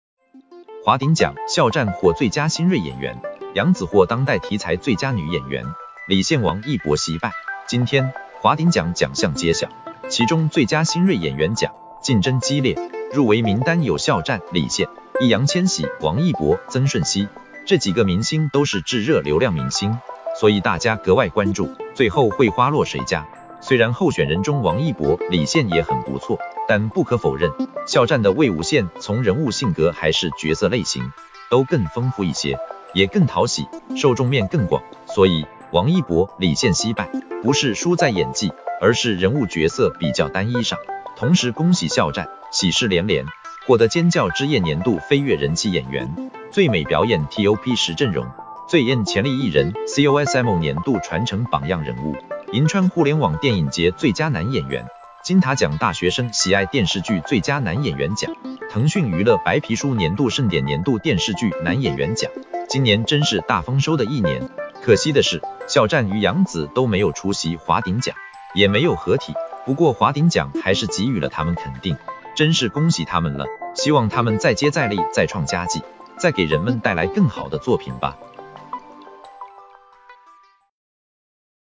华鼎奖：肖战获最佳新锐演员,杨紫获当代题材最佳女演员,李现王一博惜败
➤Thanks收看！